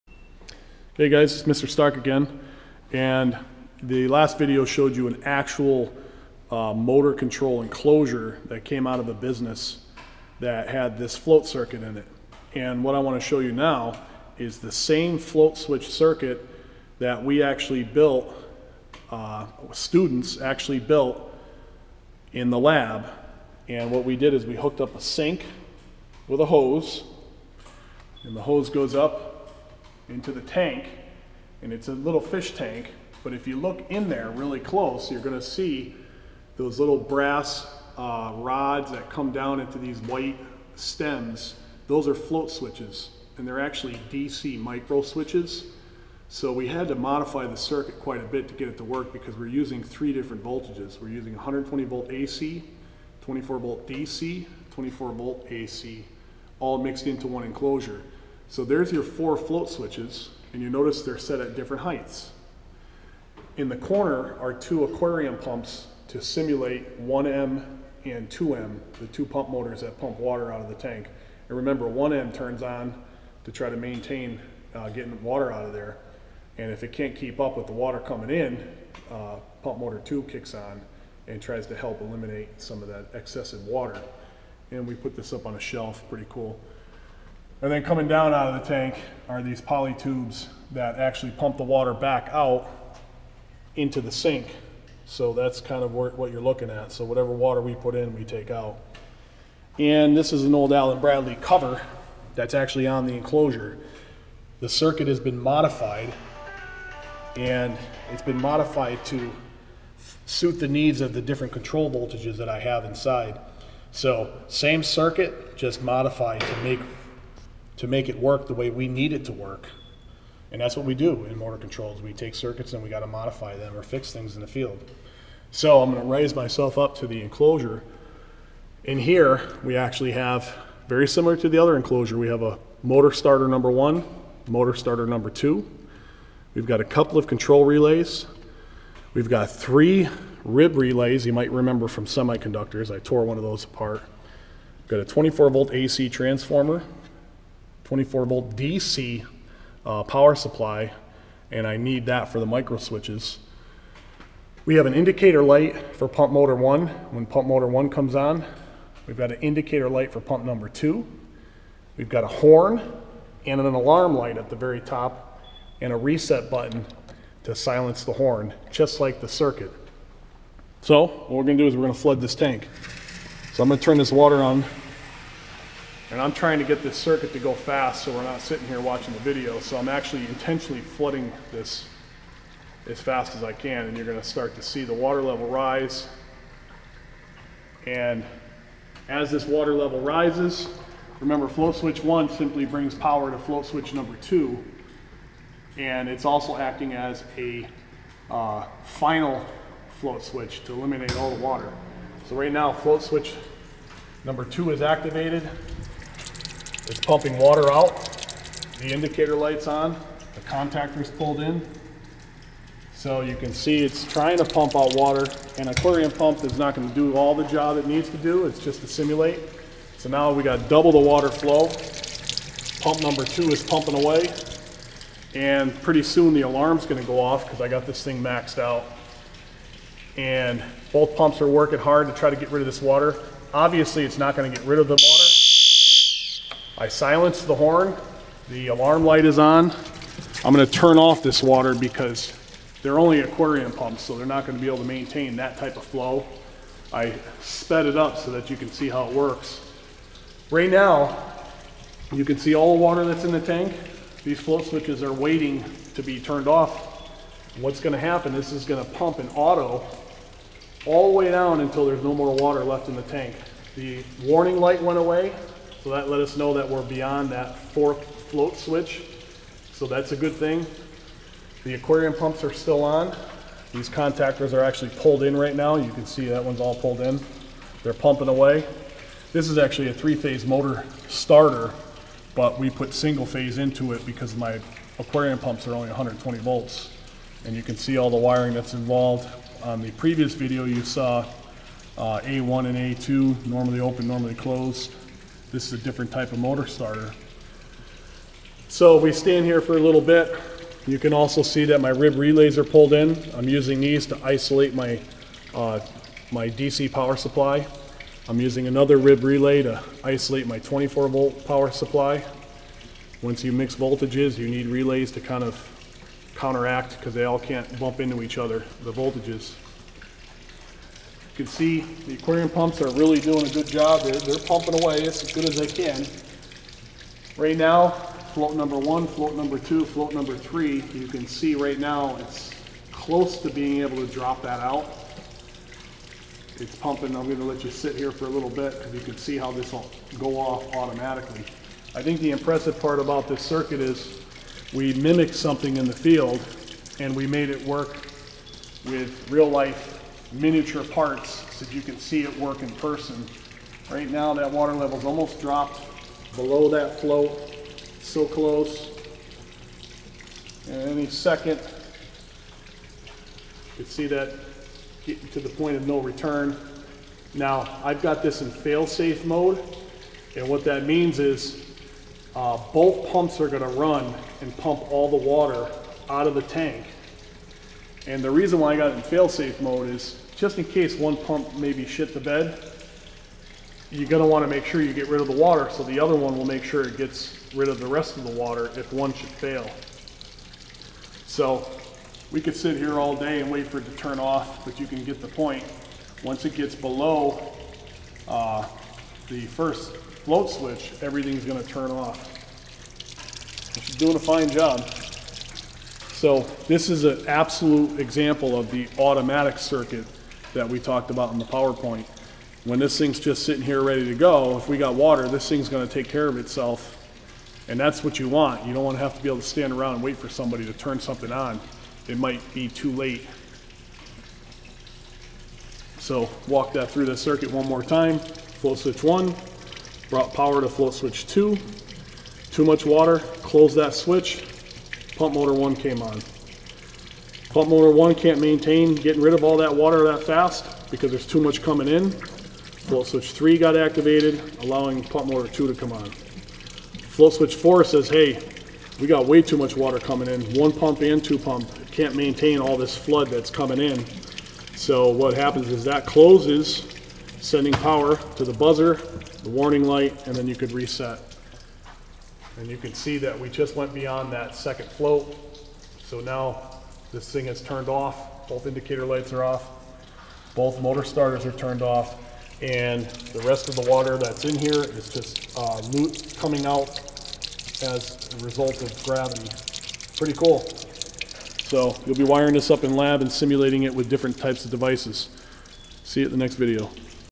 HOMEMADE PUMP CONTROLLER WITH A VARIETY OF PARTS BY MR. STARK
Miniature tank of 4 float switches with spare parts.